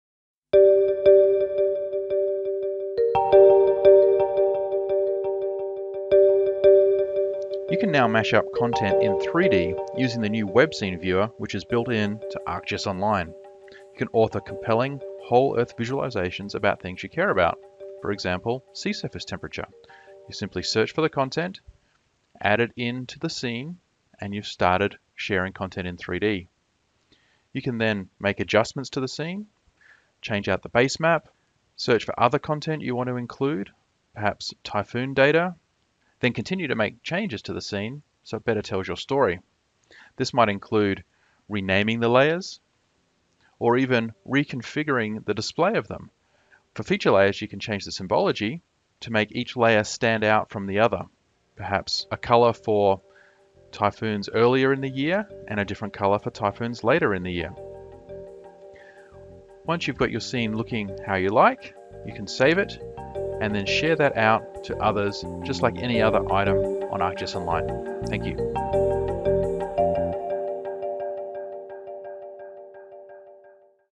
Mashup Web Scenes using ArcGIS Online
Mashup 3D content using ArcGIS Online. Combine and view content in 3D using the WebScene viewer in ArcGIS online. Add content into the scene and change the scene as needed to tell your story. Save your 3D map view and share it online.